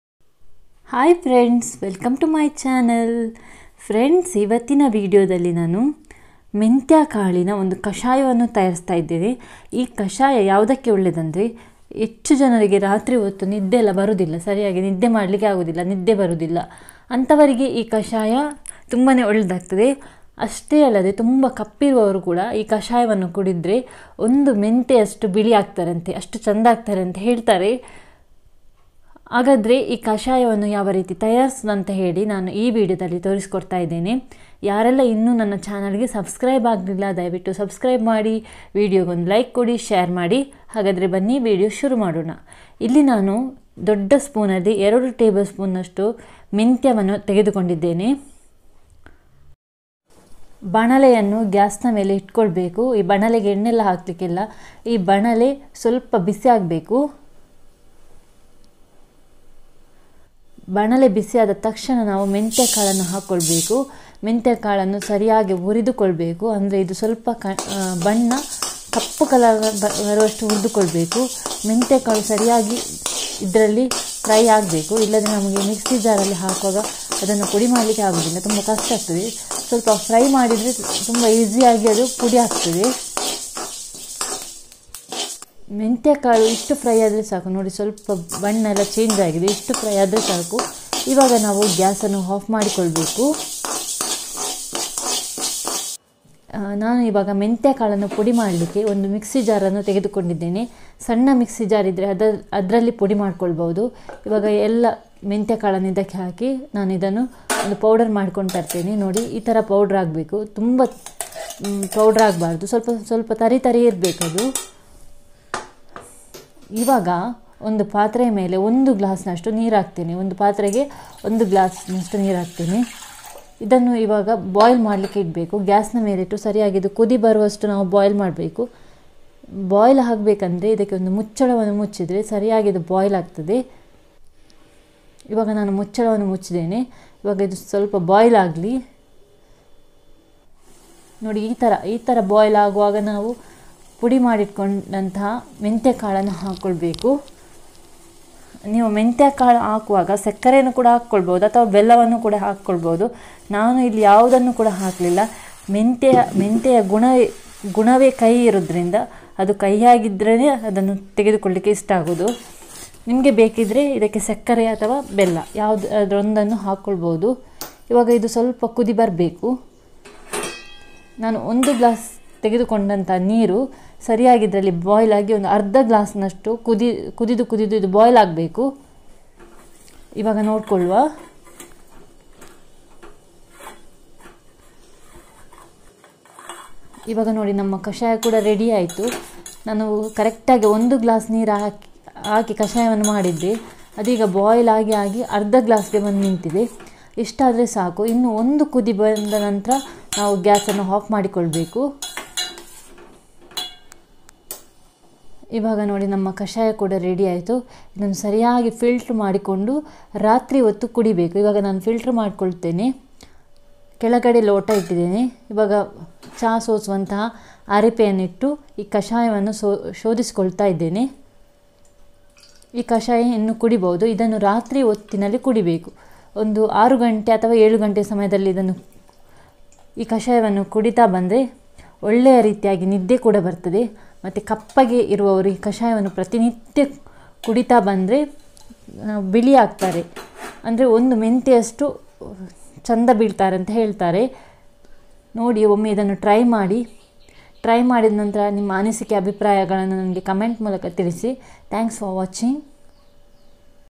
ರಾತ್ರಿ ಹೊತ್ತು ನಿದ್ರೆ ಬಾರದವರು ಈ ಕಷಾಯ ಕುಡಿಯಿರಿ/Fenugreek seeds kashya/ಮೆಂತ್ಯ ಕಷಾಯ
ರಾತ್ರಿ ಹೊತ್ತು ನಿದ್ರೆ ಬಾರದವರು ಈ ಕಷಾಯ ಕುಡಿಯಿರಿ/Fenugreek seeds kashya/ಮೆಂತ್ಯ ಕಷಾಯ@divyahomekitchen

Hi frnds